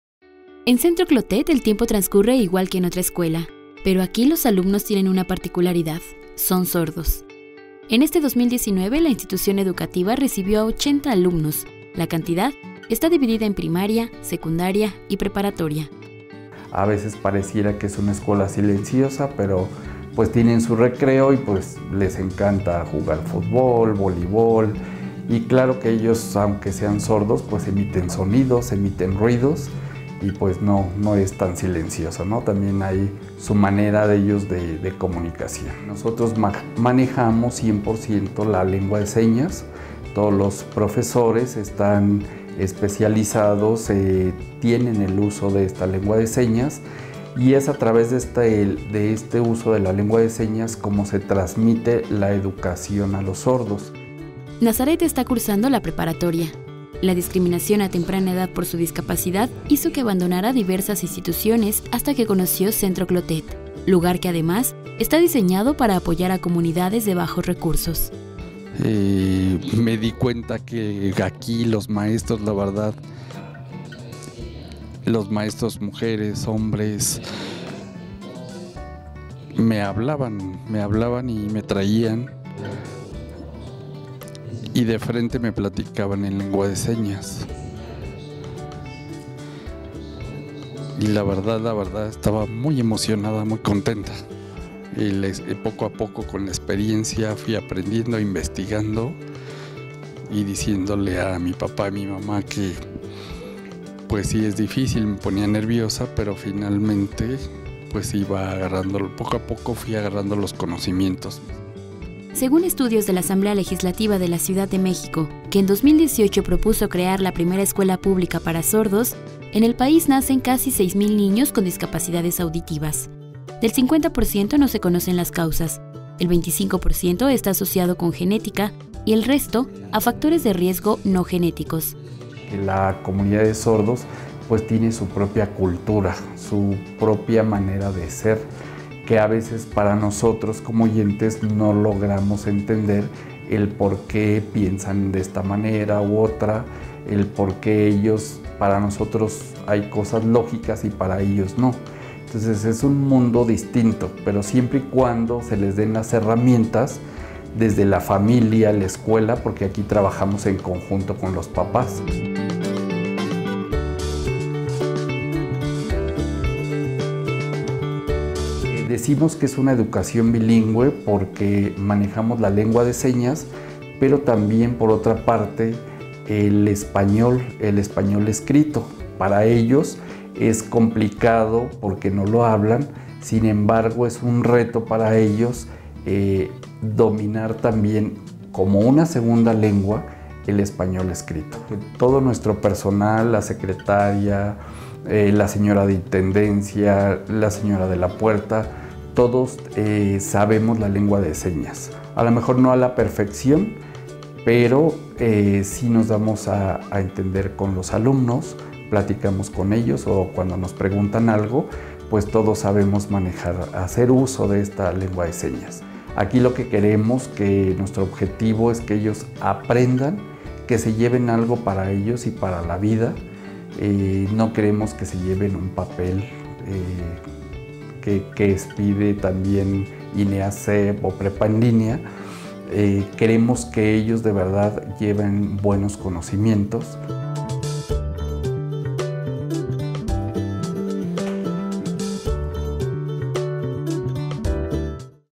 Día Nacional del Sordo: rompiendo las barreras de la educación
El Centro Clotet ofrece servicios educativos a la comunidad sorda. Ellos brindan educación primaria, secundaria y preparatoria.